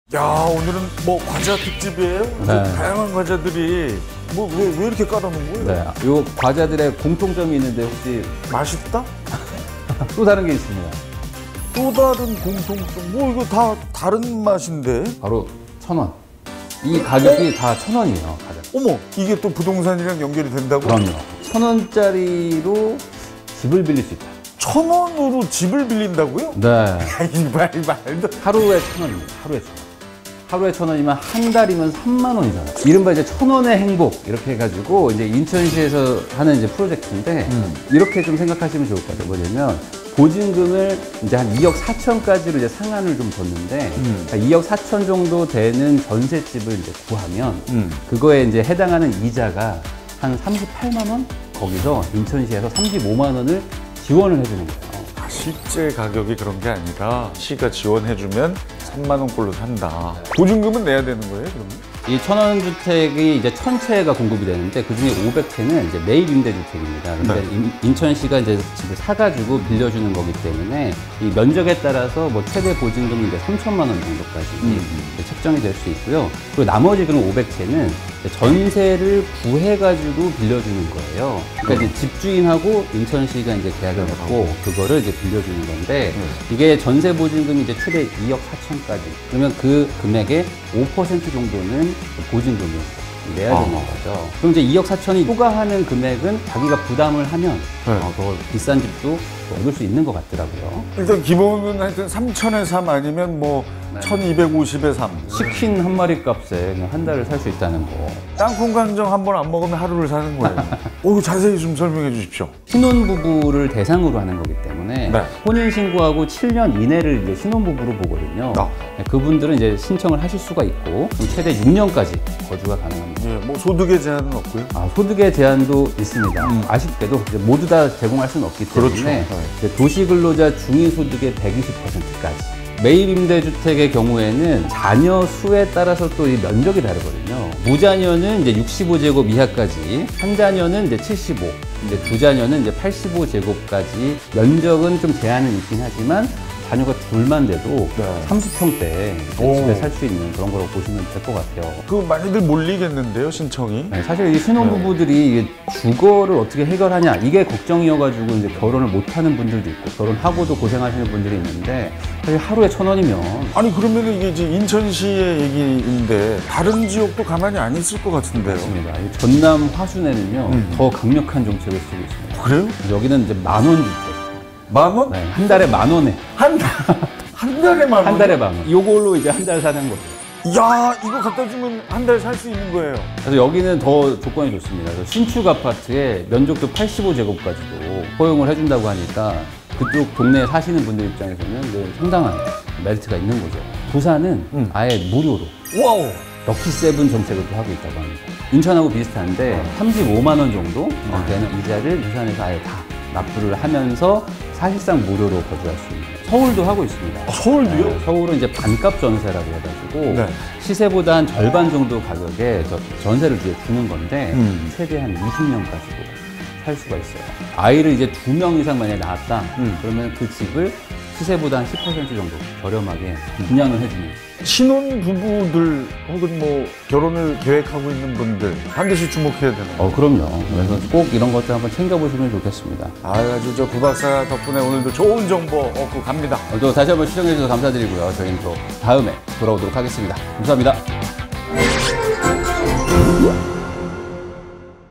노른자 식땅 EP.28 천원 주택의 행복
EP. 28 천원 주택의 행복

가성비 ‘갑’으로 떠오른 천 원짜리 과자처럼
하루에 천 원으로 빌릴 수 있는 집이 있다면?

신혼부부라면 누구든지 단돈 1,000원!!
임대 주택을 제공해 주는 인천시 '천원 주택'의
개요와 신청 기준을 알려드립니다.

(+서울시 '반값 전세', 화순 '만원 주택'의 정보도 놓치지 마세요🤫)

대한민국은 부동산 전성시대,
부동산에 대한 맛있는 이야기가 노른자 식땅에서 펼쳐집니다.